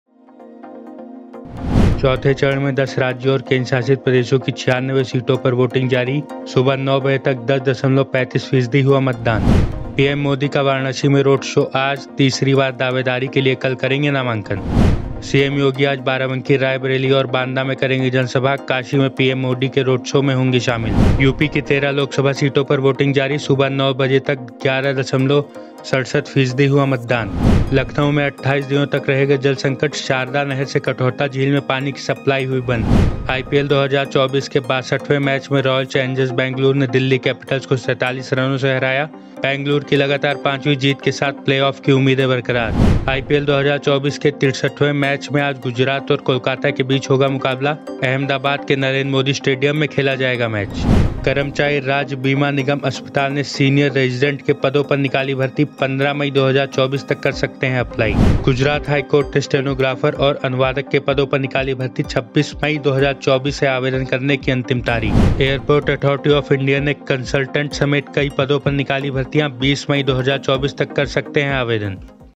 आज के सबसे बड़े समाचार | UP की ताज़ा खबरें | Baten UP Ki
1-चौथे चरण में 10 राज्यों और केंद्र शासित प्रदेशों की 96 सीटों पर वोटिंग जारी, सुबह 9 बजे तक 10.35 फीसदी वोटिंग, बंगाल में सबसे ज्यादा 15.24 फीसदी मतदान

2- पीएम मोदी का आज वाराणसी में रोड शो, तीसरी बार दावेदारी के लिए कल करेंगे नामांकन

3-सीएम योगी आज बाराबंकी, रायबरेली व बांदा में करेंगे जनसभा, काशी में पीएम मोदी के रोड शो में भी होंगे शामिल

4-यूपी की 13 सीटों पर वोटिंग जारी, सुबह 9 बजे तक 11.67% मतदान, सीतापुर में सबसे अधिक तो शाहजहांपुर में सबसे कम

5-लखनऊ में 28 दिनों तक रहेगा जल संकट, शारदा नहर से कठौता झील में पानी की सप्लाई बंद

6- आईपीएल 2024 के 62वें मैच में रॉयल चैलेंजर्स बेंगलुरु ने दिल्ली कैपिटल्स को 47 रनों से हराया, बेंगलुरु की लगातार पांचवीं जीत के साथ प्लेऑफ की उम्मीदें बरकरार

7-IPL 2024 के 63वें मैच में आज गुजरात और कोलकाता के बीच होगा मुकाबला, शाम 7:30 बजे से अहमदाबाद के नरेंद्र मोदी स्टेडियम में खेला जाएगा मैच

8-कर्मचारी राज्य बीमा निगम अस्पताल (ESIC) ने सीनियर रेजिडेंट के पदों पर निकाली भर्ती, 15 मई 2024 तक कर सकते हैं अप्लाई

9-गुजरात हाईकोर्ट ने स्टेनोग्राफर और अनुवादक की निकाली भर्ती, 26 मई 2024 है आवेदन की अंतिम तारीख

10-एयरपोर्ट अथॉरिटी ऑफ इंडिया (AAI) ने कंसल्टेंट, जूनियर कंसल्टेंट और एसोसिएट कंसल्टेंट के पदों के लिए निकाली भर्तियां, 20 मई 2024 तक कर सकते हैं अप्लाई

धन्यवाद